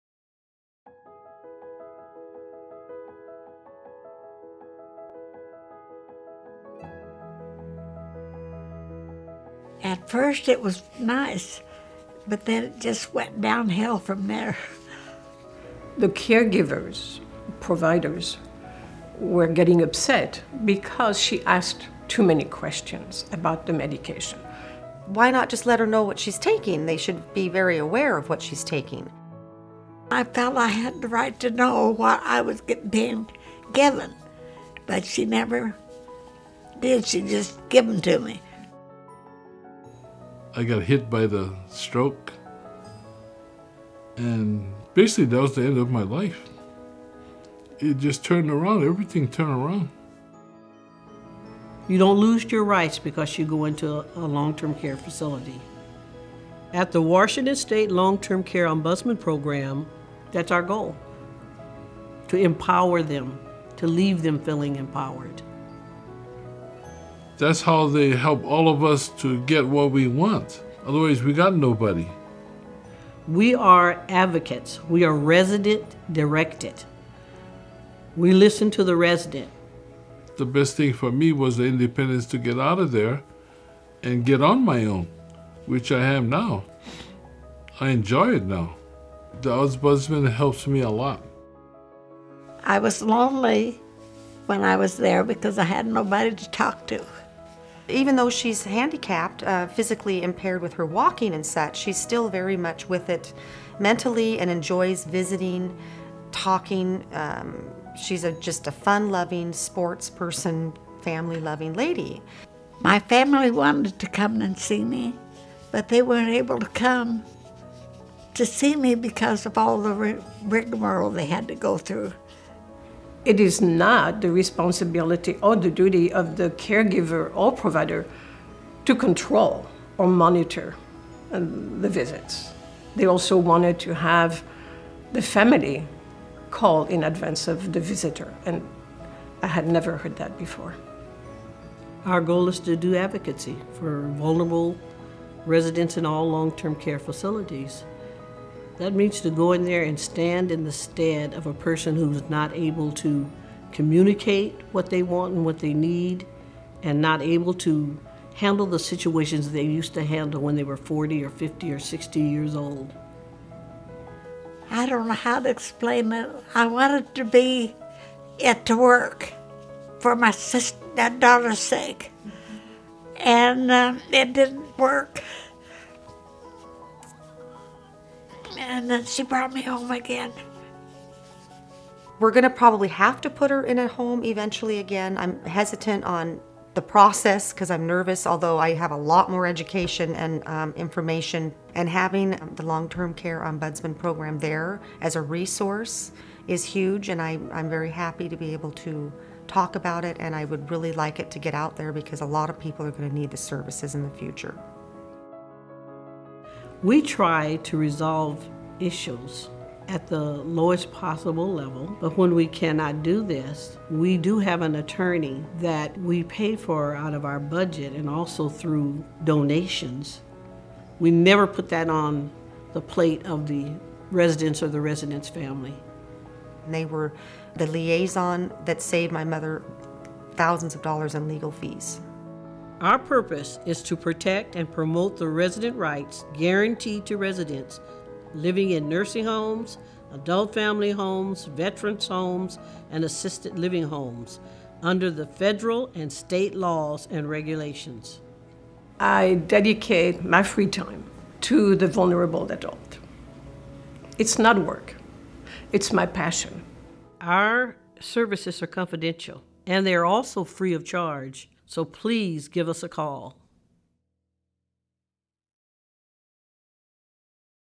WA State Long Term Care Ombudsman Program Overview
Overview of the Washington State Long-Term Care Ombudsman Program. We work to promote and protect residents' rights.